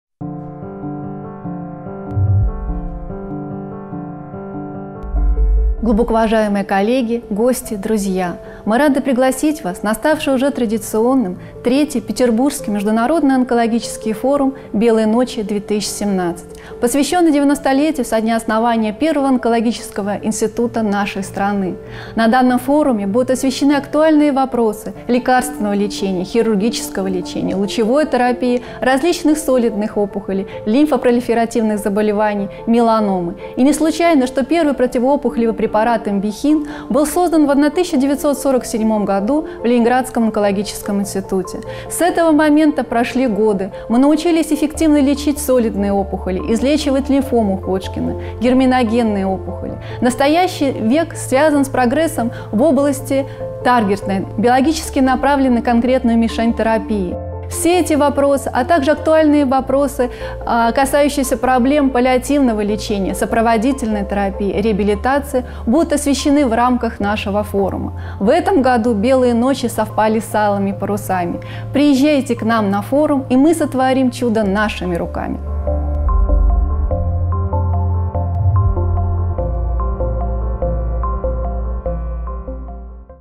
Татьяна Юрьевна Семиглазова - Белые ночи 2017
Приглашение к участию в III-ом Петербургском международном онкологическом Форуме "Белые ночи - 2017"
Благодарим компанию за оказанную поддержку